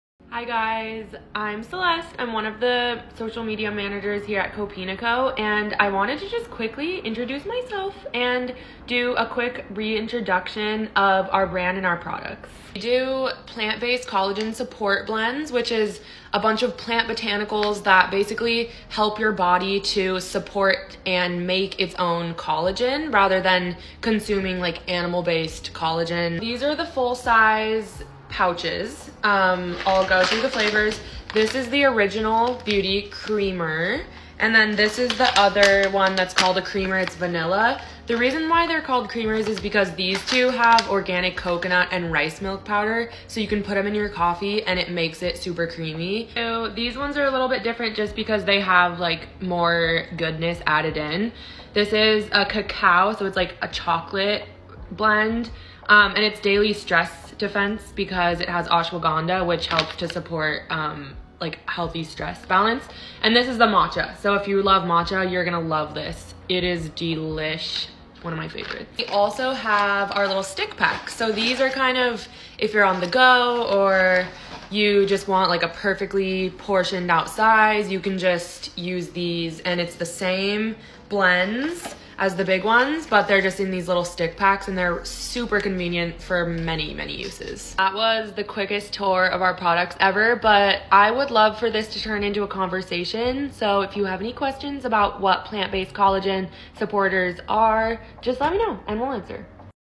Introducing... Vegan Collagen Supports!
A very quick tour of our products and brand. Taking collagen supplements is amazing because we create less collagen as we age, and adding it back in can actually decrease signs of aging by preserving the health of your skin, hair, and nails!